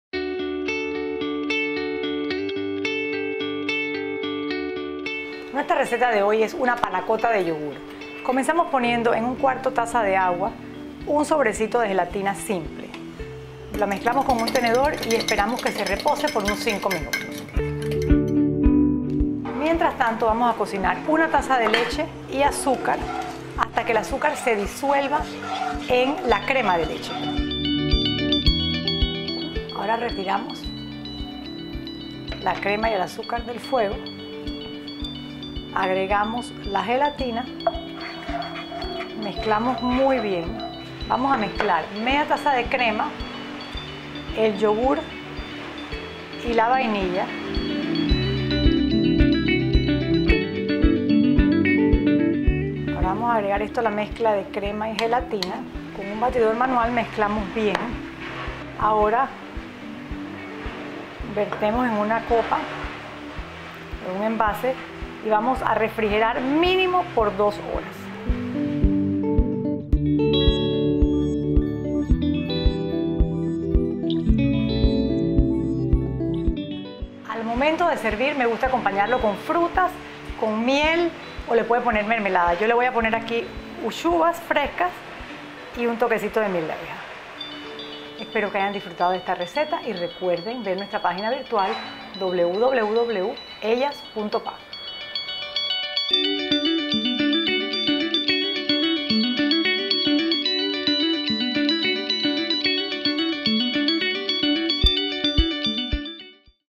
Ellas.pa: Cocina - ‘Panna cotta' de yogur
Un postre excelente para el verano. Lleva crema de leche, gelatina y yogur natural estilo griego.